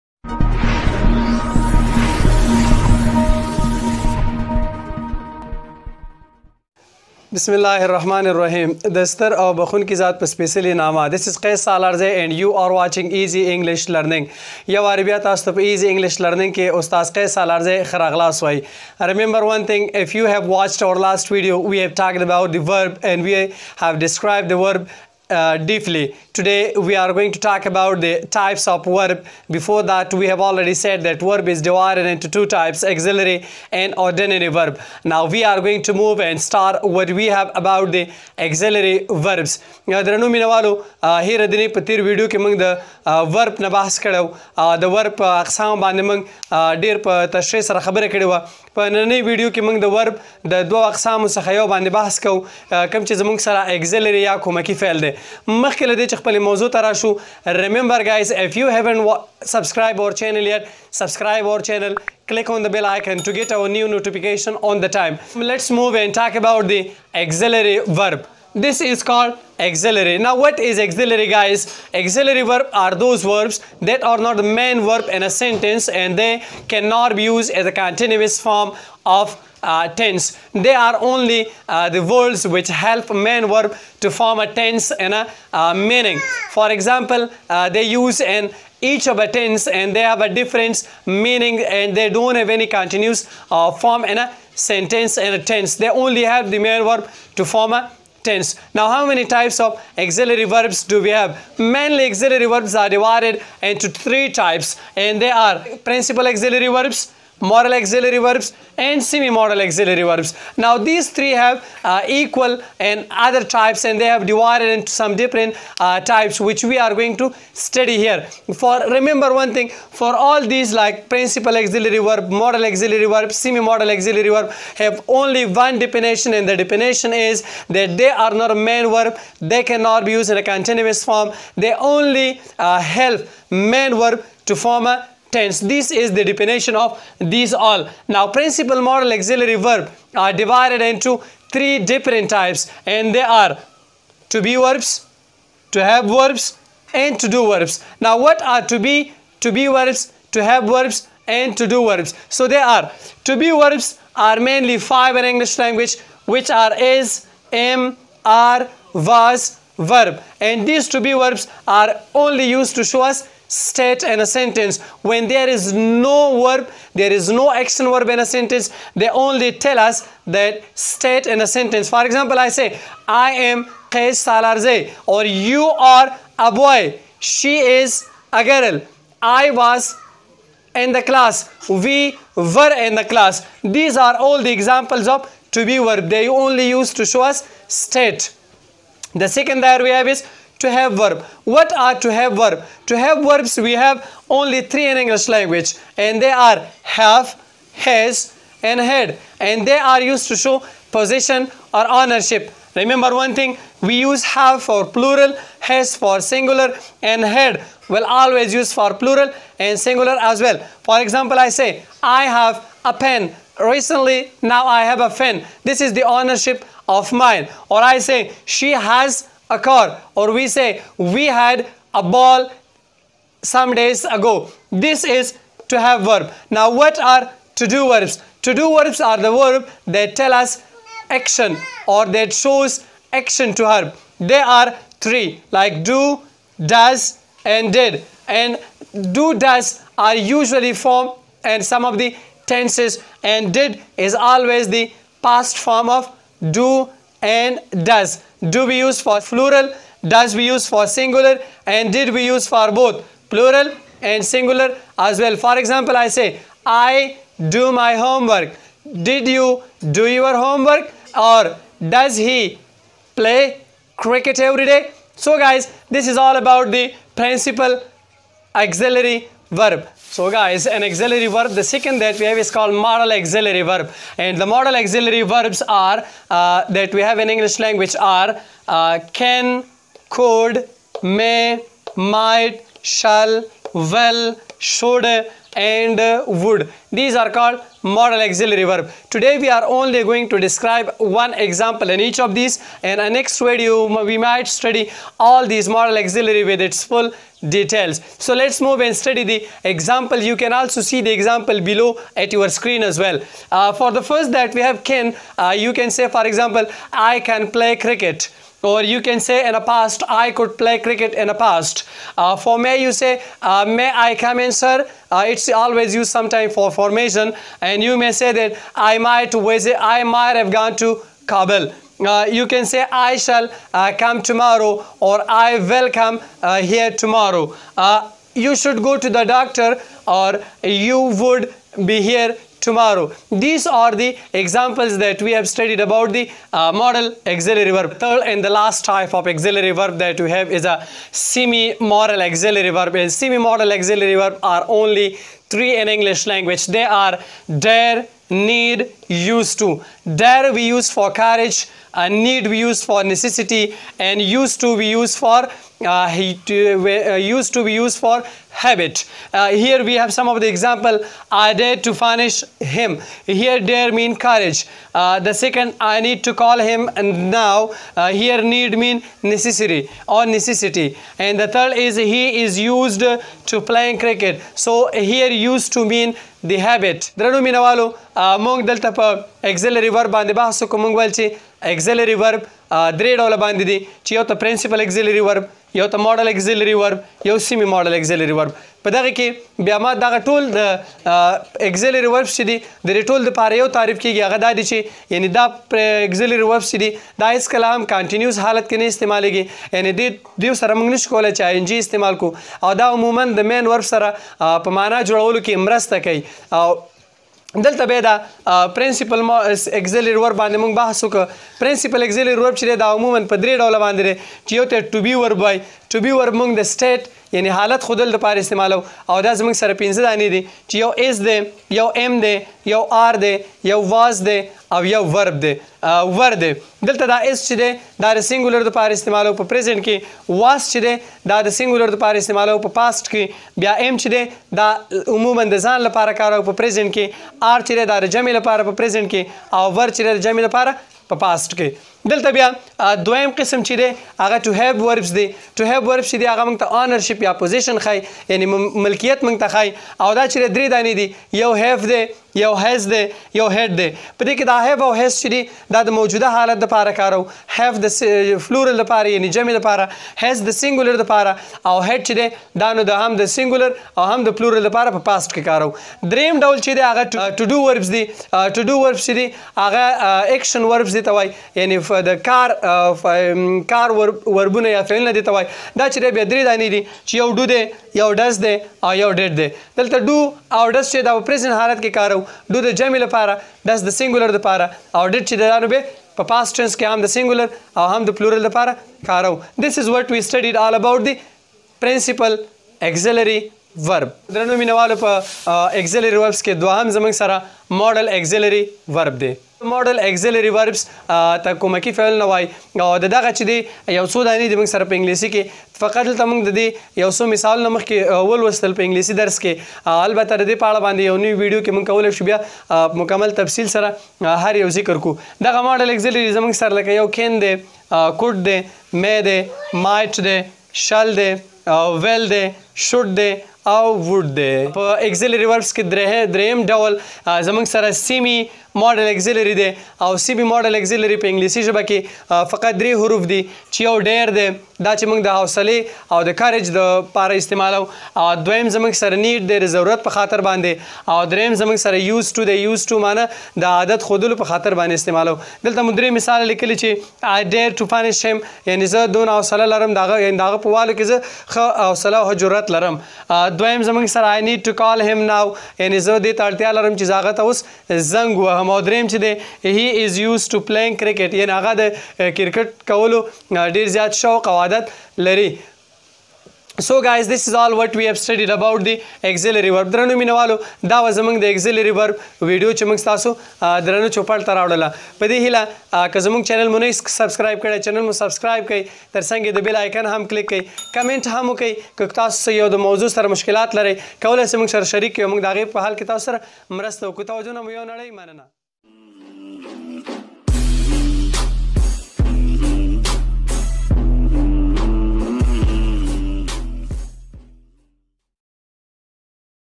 Verb | types of verb | Auxaliary verb | grammer | qais salarzai
Hi dear friends welcome back to Easy English Learning. Today we have brought to you the remaing part of Verb. In which we are talking all about the Auxiliary verbs which are also known as helping verbs. After watching this video we are sure that you will solve all your problems related with verbs and hope to enjoy speaking English.